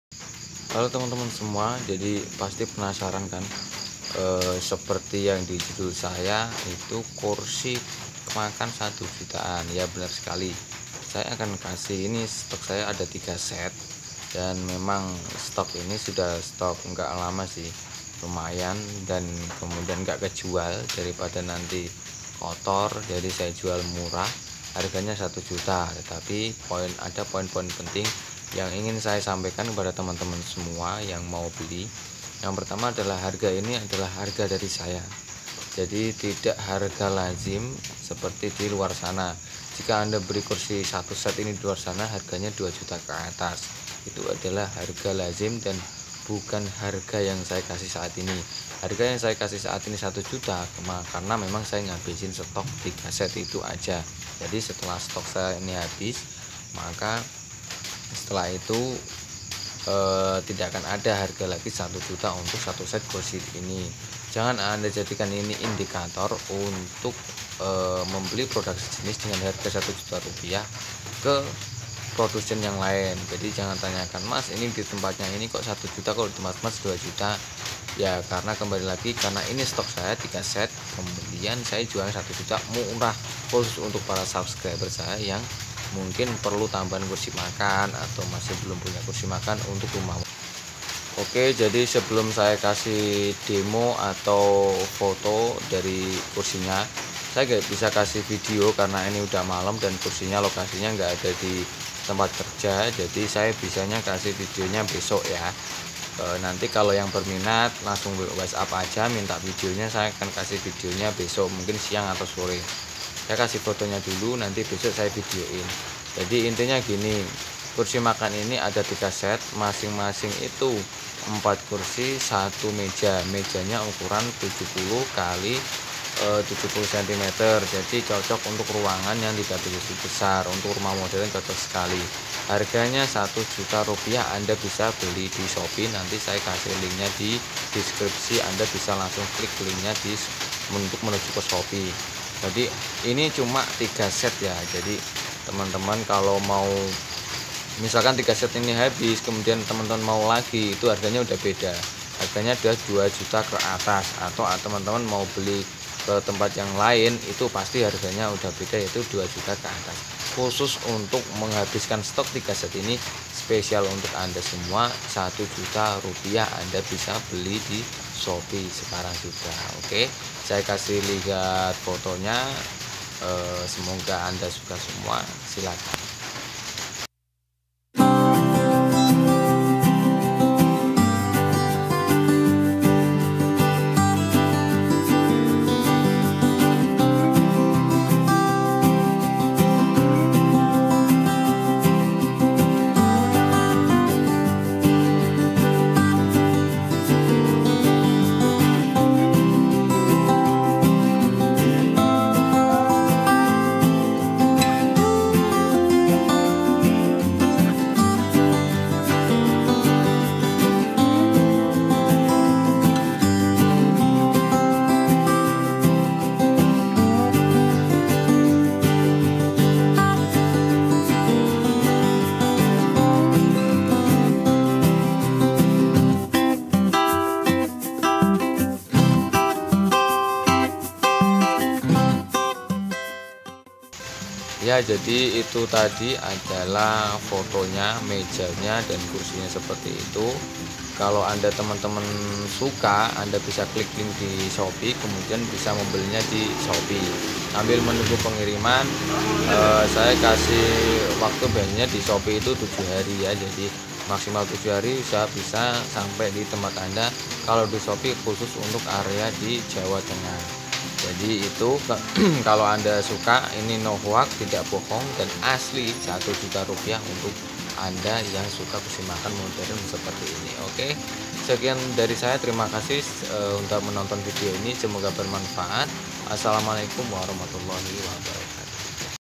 STOK TERBATAS‼ Meja Dan Kursi Makan Modern 1 SET 1 JUTA SAJA
NO HOAX!! Stock tinggal 3 set. Per set nya harga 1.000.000 Bisa beli di shopee. MEJA KURSI MAKAN MINIMALIS SPESIFIKASI SUDAH SAYA JELASKAN DI VIDEO MOHON DITONTON SAMPAI SELESAI SUPAYA LEBIH JELAS.